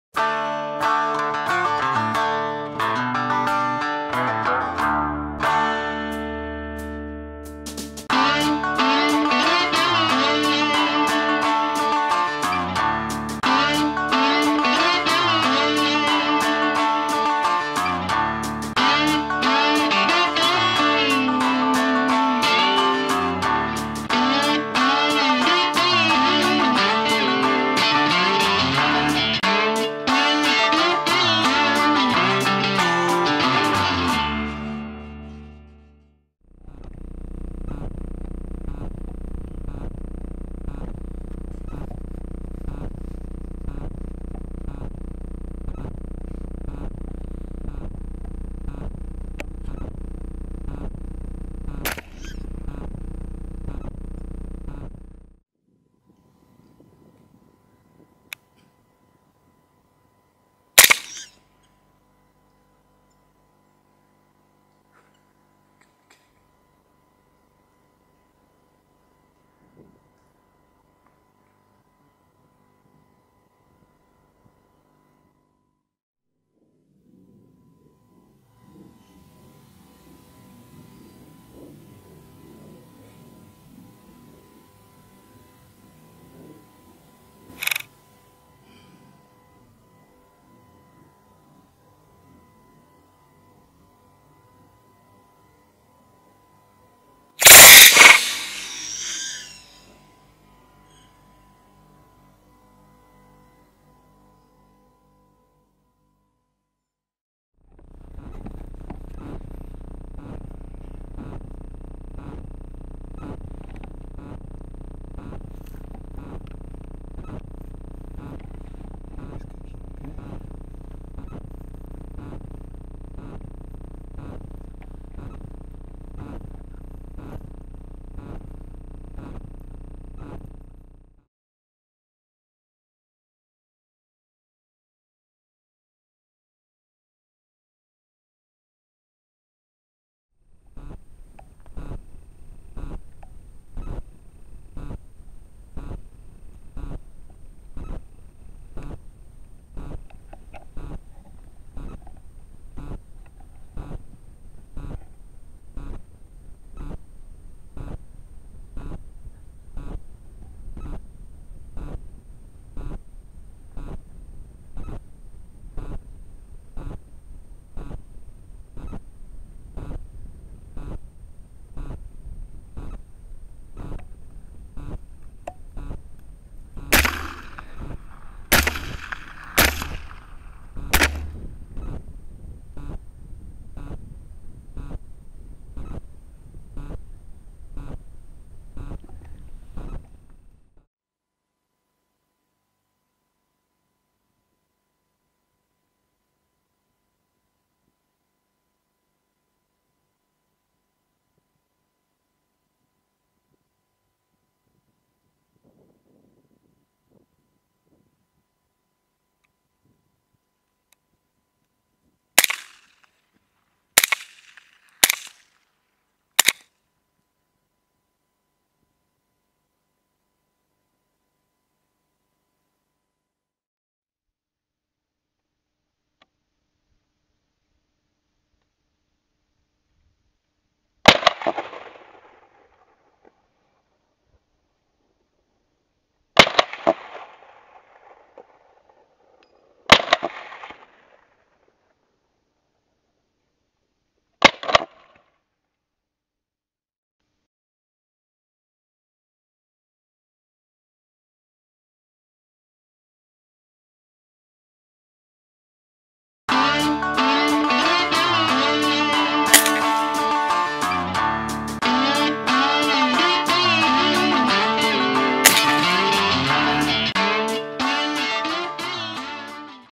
ATN X-Sight: Texas Razorback Boar (Actual Footage)
Video footage recorded with ATN X-Sight HD 3-12x Day/Night Riflescope.